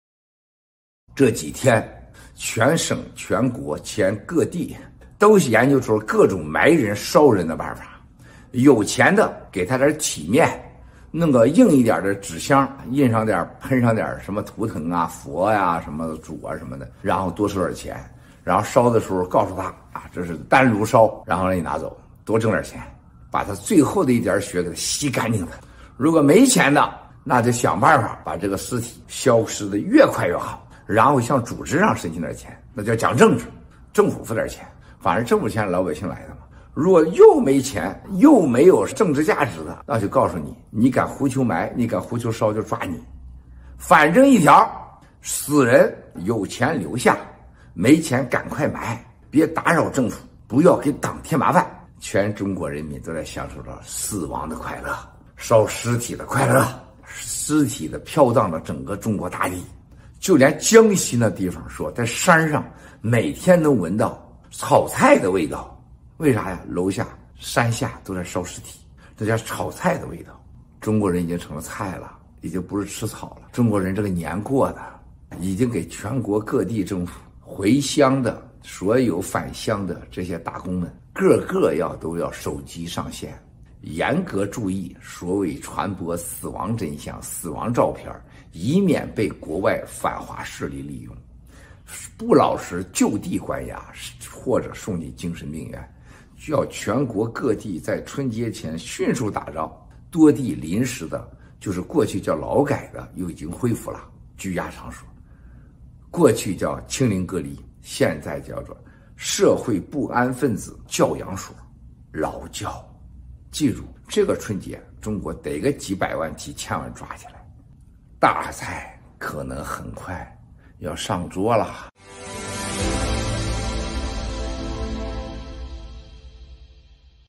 2023.01.23 In the past few days, the CCP has been inventing ways of the disposal of bodies all
over the country, and trying every means to suck the last penny from ordinary people. The CCP has asked local governments to monitor returnees, and strictly pay attention to the so-called photos that spread the truth about the deaths to prevent them from being used by "foreign anti-China forces". Before the Chinese New Year, all around China was asked to quickly build temporary "re-education through labour camps". Tens of millions of people will get arrested during this Chinese New Year holiday. The big event may be taken place soon.
这几天CCP在全国各地研究出各种处理尸体的办法，想尽一切办法得从老百姓身上吸干最后一分钱。CCP已经让各地政府监控返乡人员，严格注意所谓传播死亡真相死亡照片，以免被“国外反华势力”利用。还要全国各地在春节前迅速打造劳改拘押场所，这个春节中国得几百万几千万人抓起来，大菜可能很快要上桌啦